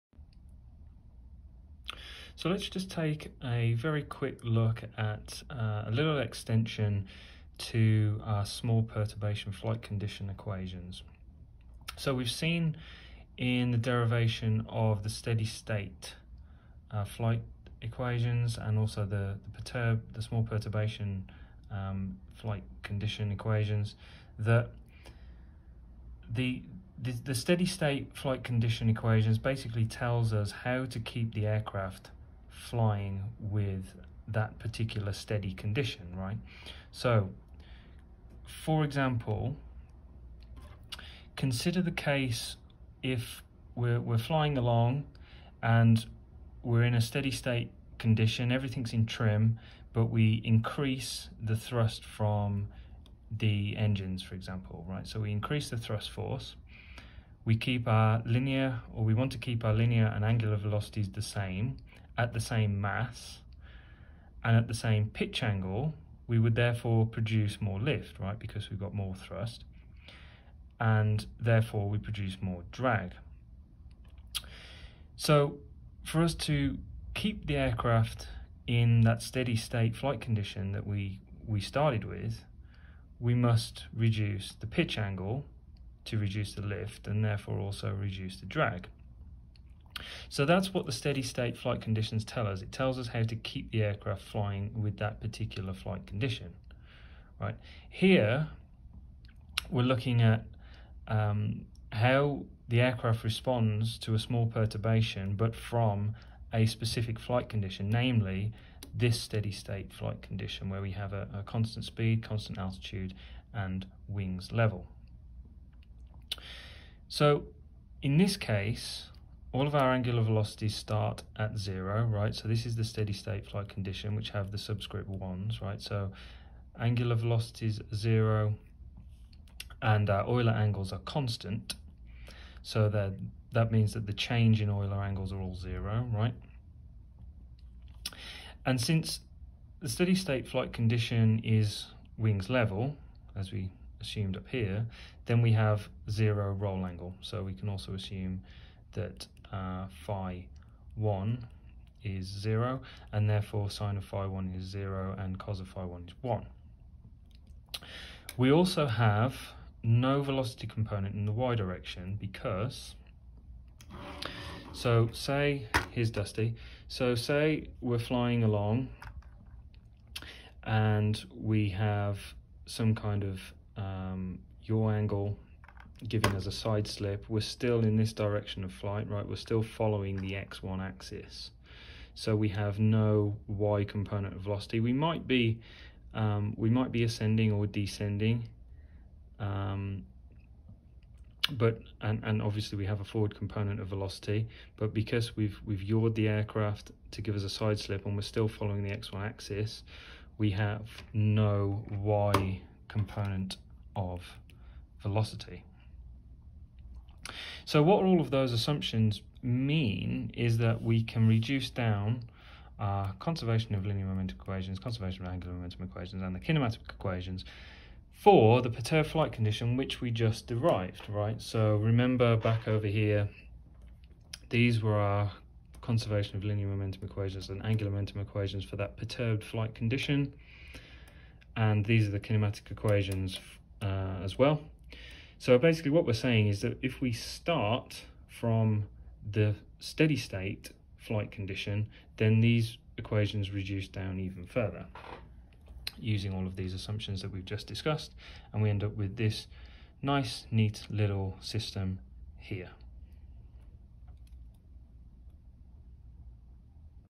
1.10 Perturbed Flight Conditions from a Steady State Condition
This video describes how the perturbed flight condition equations may be reduced if the perturbation is considered from a steady state flight condition.